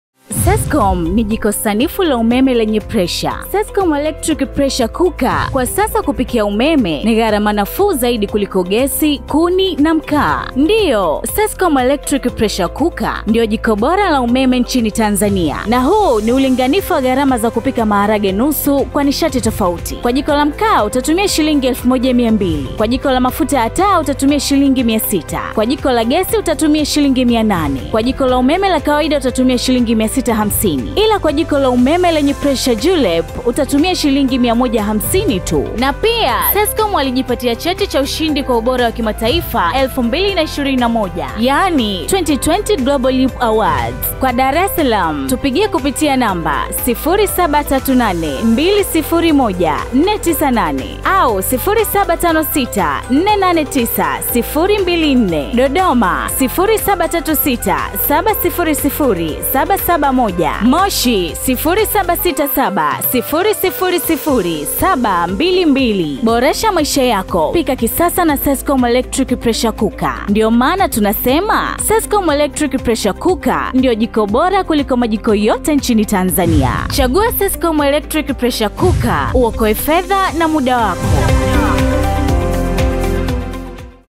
SESCOM ELECTRIC PRESSURE COOKER (SEPC)
SESCOM Electric Pressure Cooker is one of the efficient electrical cooking appliance which consumes less energy during cooking as compared to LPG or charcoal. It can cook 1 kg of beans for one hour using energy of  0.4 Kwh which is equivalent to TZS 140.  The Global LEAP Awards Announced SESCOM Electric Pressure Cooker a winner for the 2020 Electric Pressure Cooker Competition.  This unique competition has evolved into a trusted global brand that serves as the de facto source of accurate, actionable information about the quality and energy performance of off-grid appliances